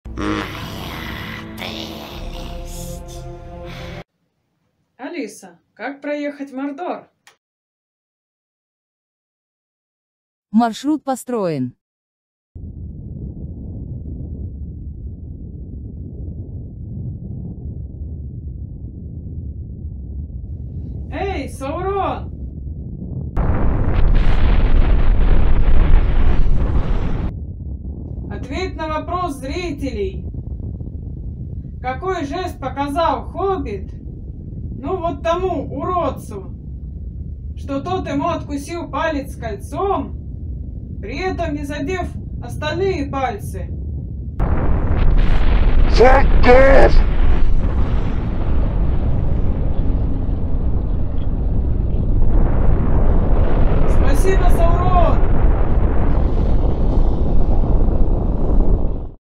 Анекдот от Толкинистов
В основе сюжета анекдот который нам рассказали Толкиенисты. А так же присутствует мем про дорогу в